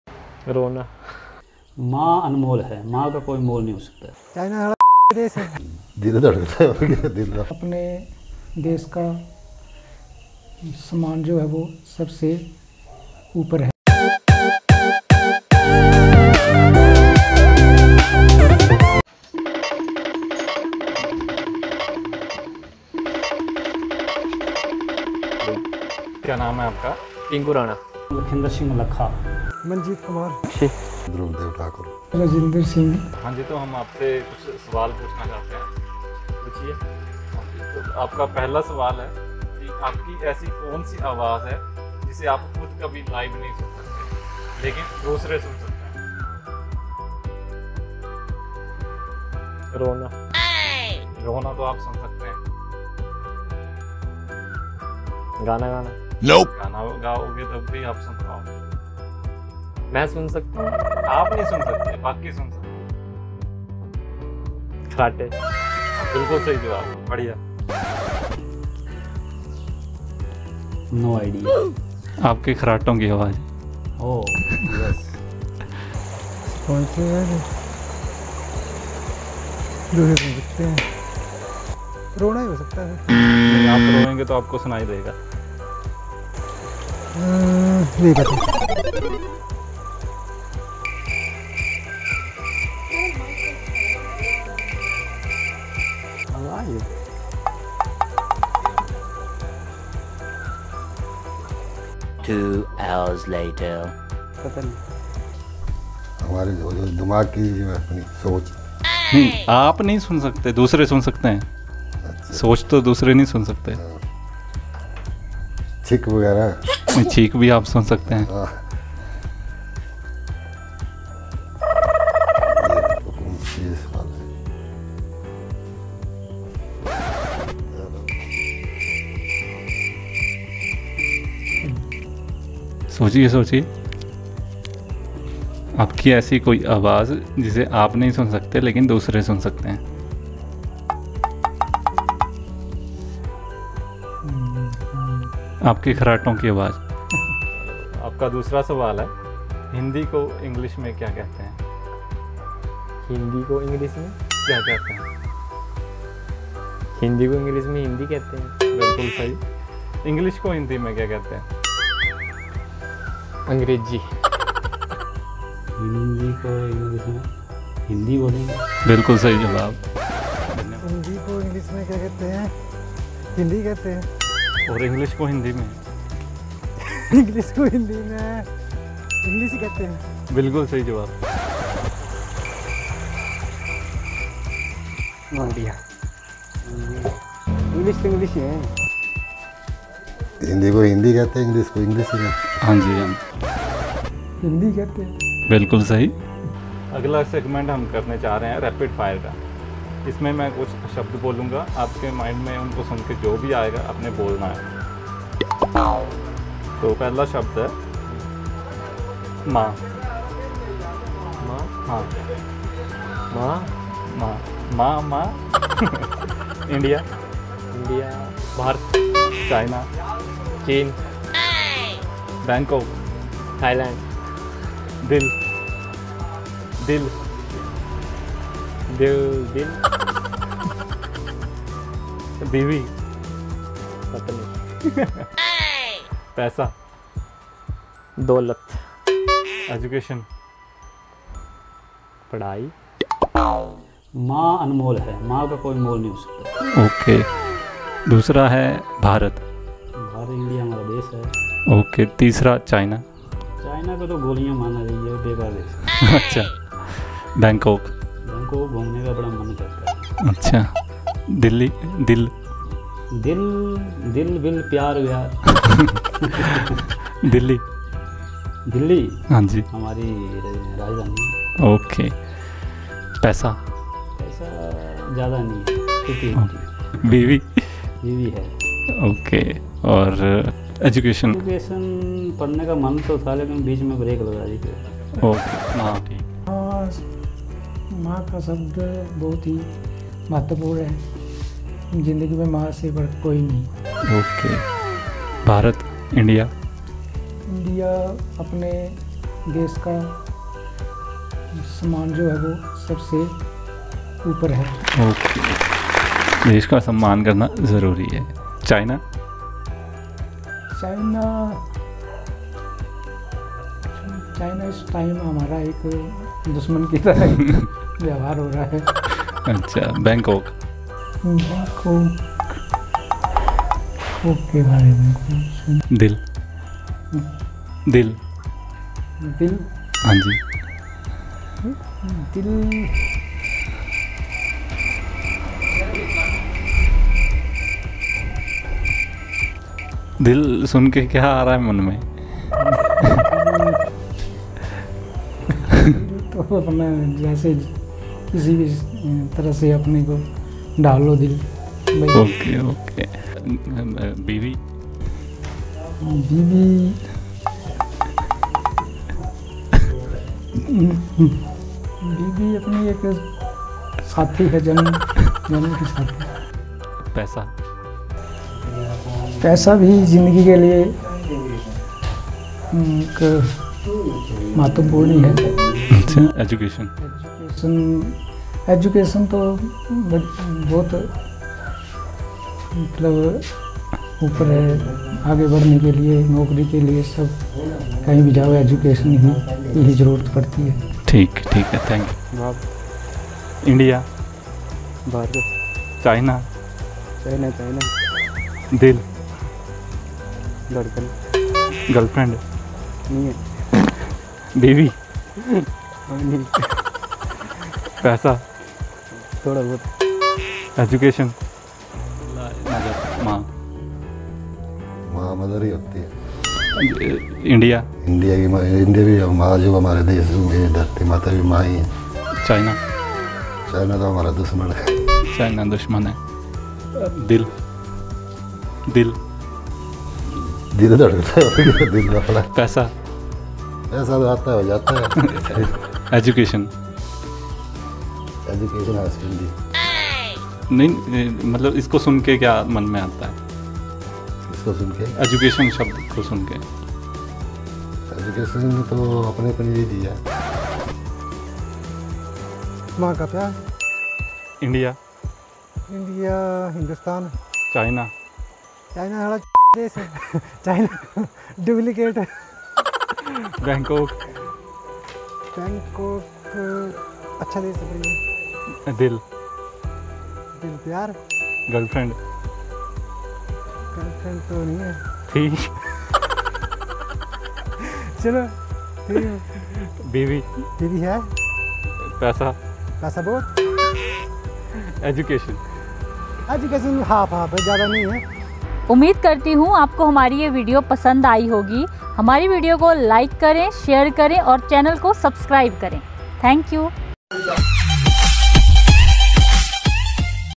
🔥Fun with Kangra Ki Public👪😍🤩🤓💯🔥Kangra Ki Janta k Sath Kuch Funny Questions🔥🤩
Is video me aapko dekhne ko milenge Kangra (Baijnath, Chadhiar) k logo k jwab  kuch common sense based funny questions k..